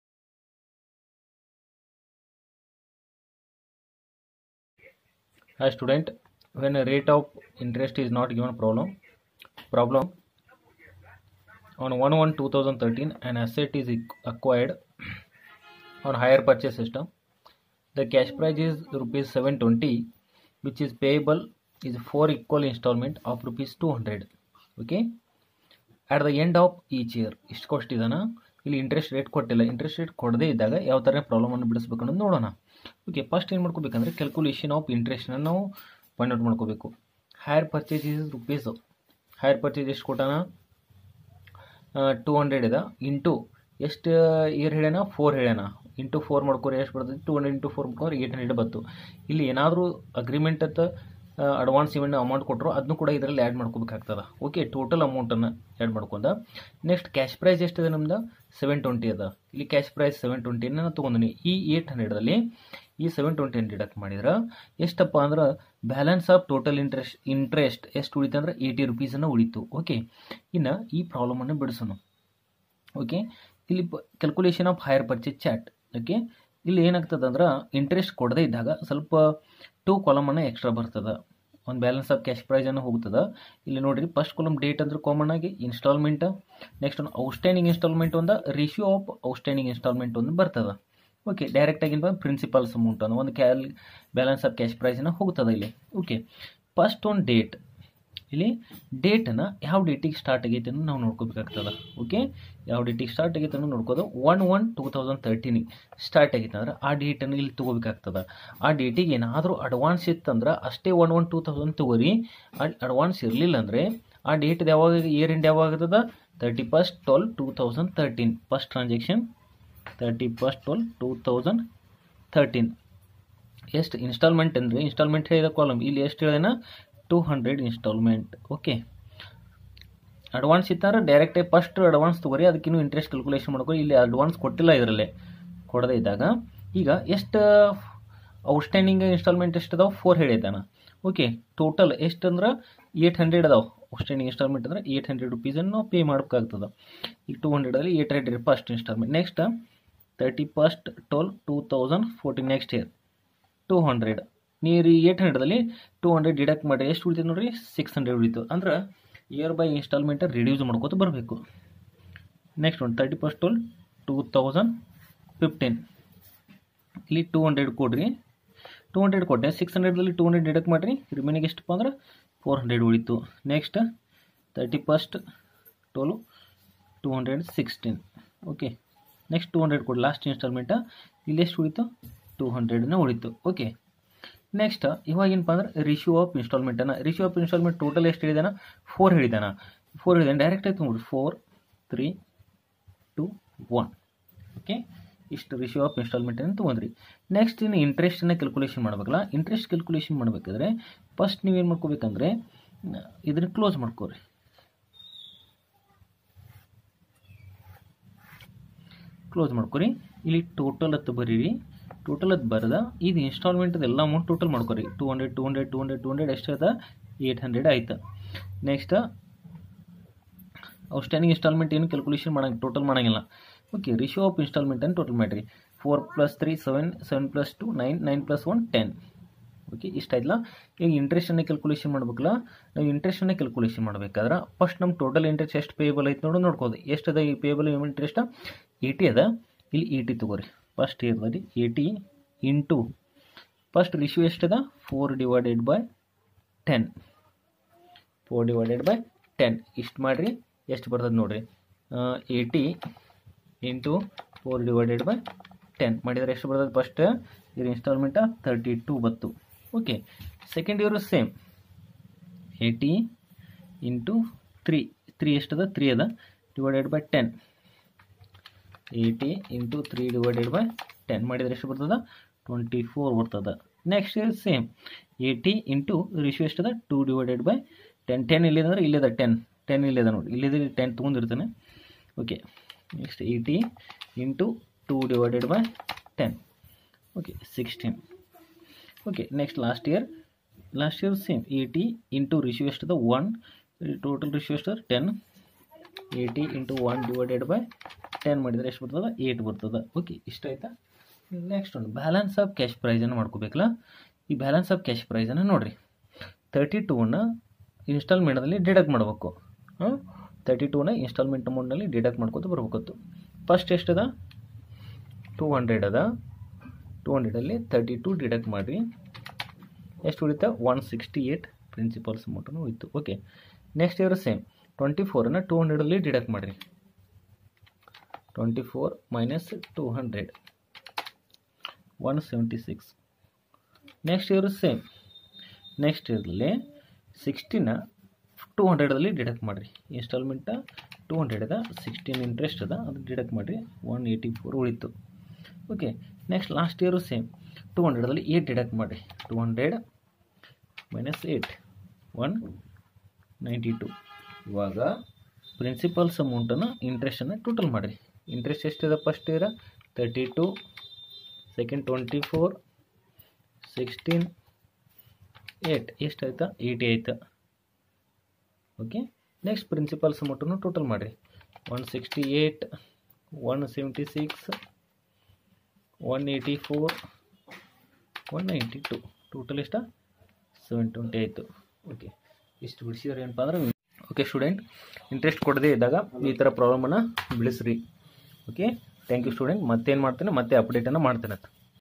Hire Purchase System | Problem & solutions 5 marks( Rate of interest are not given) Class-9
When Rate of interest are not given